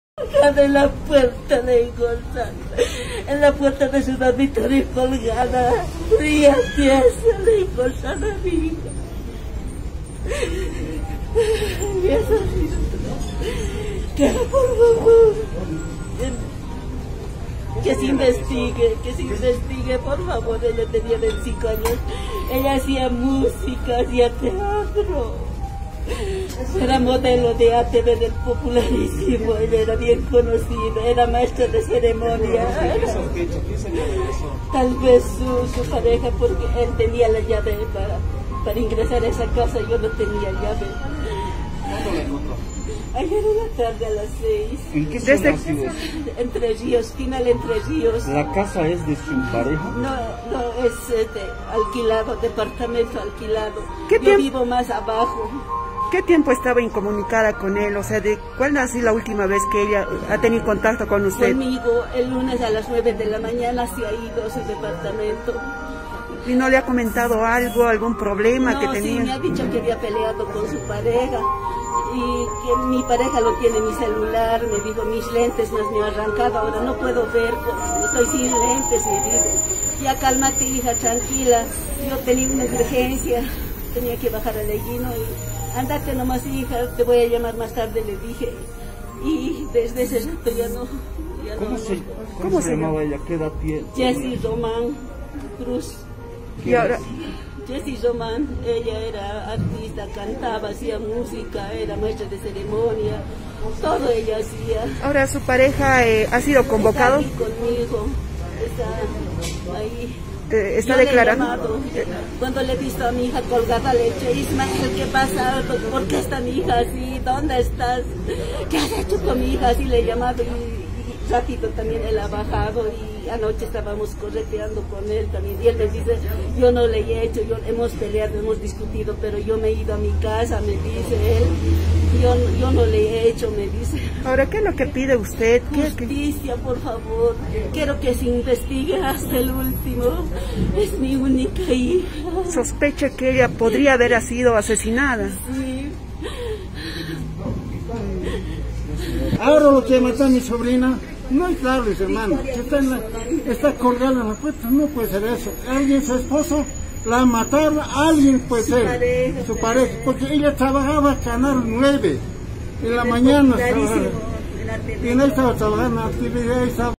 Una madre encontró sin vida a su hija,colgada en la habitación de su domicilio en la zona Entre Ríos
Una madre encontró sin vida a su hija, colgada en la habitación de su domicilio en la zona Entre Ríos de la ciudad de La Paz, se trata de Jessy Román, modelo del programa televisivo "El Popularísimo". No cree que se trate de un suicidio y sospecha de su pareja ya que días antes habrían tenido una pelea.